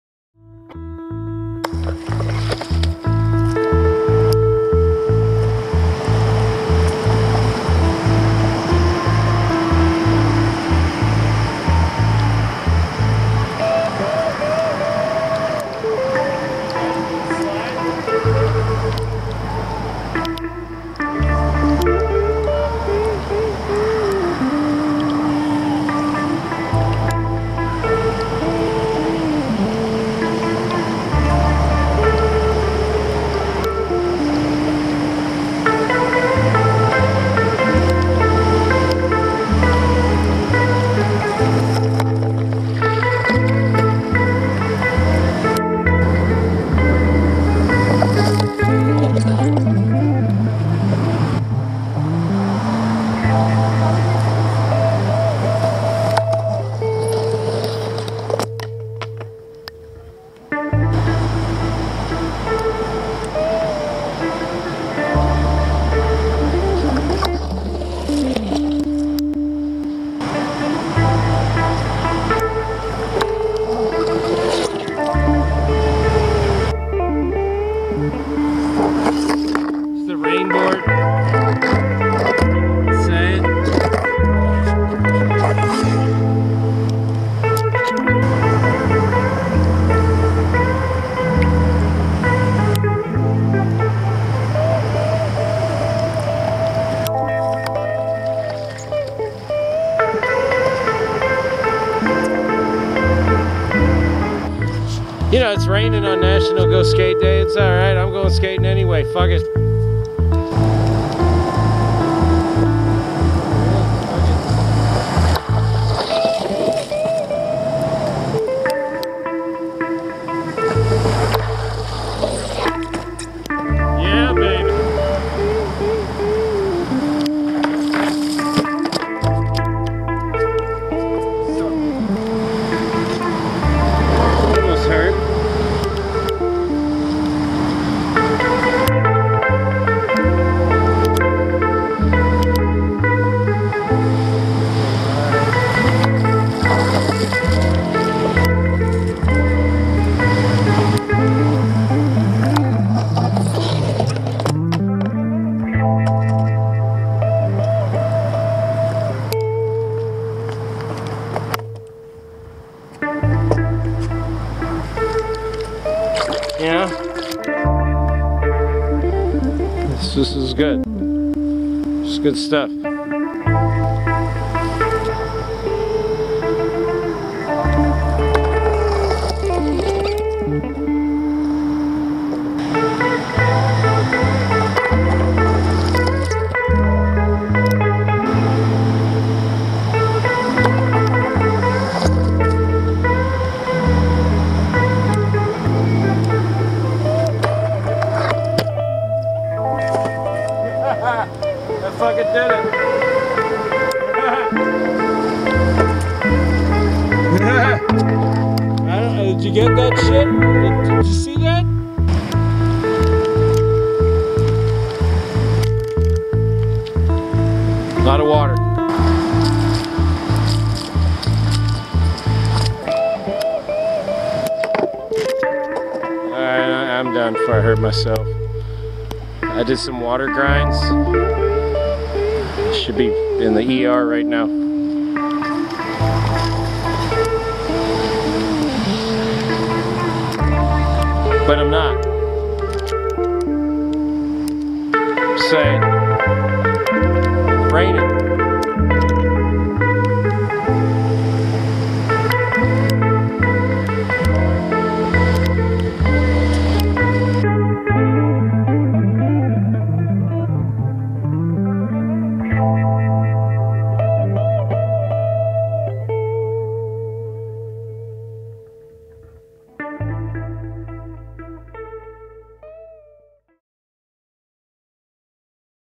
2023 go skate day in the rain Bradenton, Florida
Rain Session River Walk Skate Park Bradenton , Florida